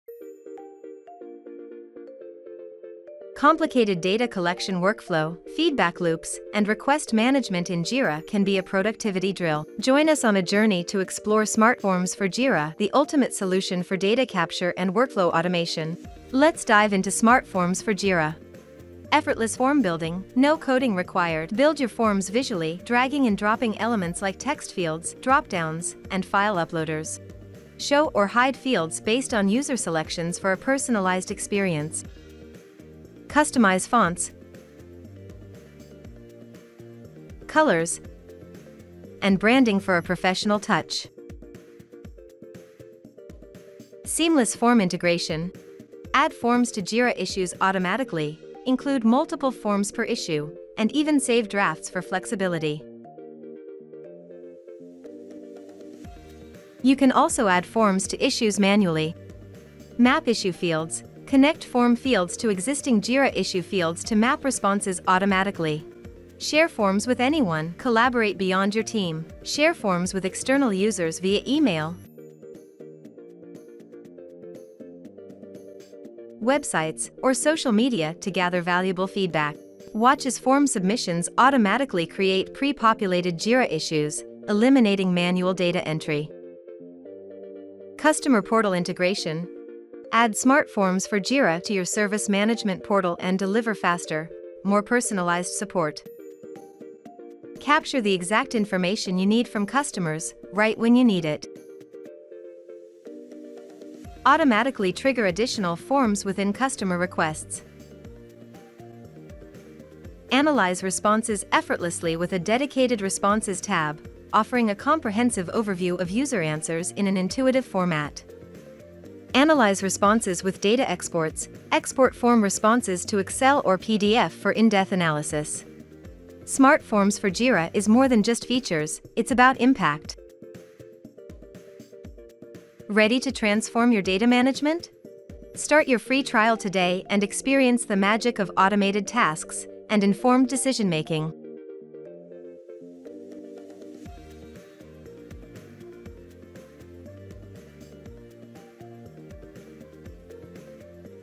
Smart Forms for Jira: Build Form to Create Jira Tickets, Collect Data, and Automate Tasks
Smart Forms for Jira Cloud: Create forms for all Jira issues

This video shows how you can use Smart Forms to create custom forms, map fields to Jira issues, and automate task generation based on form submissions. Whether you need to gather feedback, manage requests or automate workflows, these forms can be shared externally, allowing users to create Jira issues without logging into Jira.

In this video, you'll learn how to:
00:20 - How to create a form in Jira software project?
00:34 - Customize Jira full form
00:44 - Add Intake Form inside Jira issue
01:00 - Map form fields to Jira issue fields
01:06 - Share Jira Form externally to let anyone create Jira issues
01:16 - Embed Jira Forms on Confluence, Wordpress or Wix to create Jira ticket
1:30 - Add Form to Jira Service managment Customer Portal
1:50 - Analyse form responses in Jira
1:59 - Export forms from Jira to Exel or PDF

🌐Do you want more information?